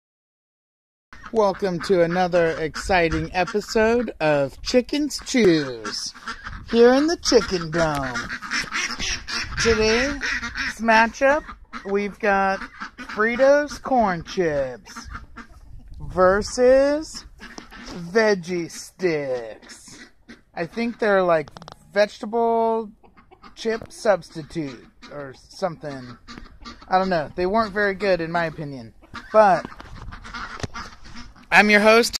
Fritos vs Veggie Sticks part 1
Fritos and Veggie Sticks square off in the Chicken Dome in this winner take all battle for food supremacy!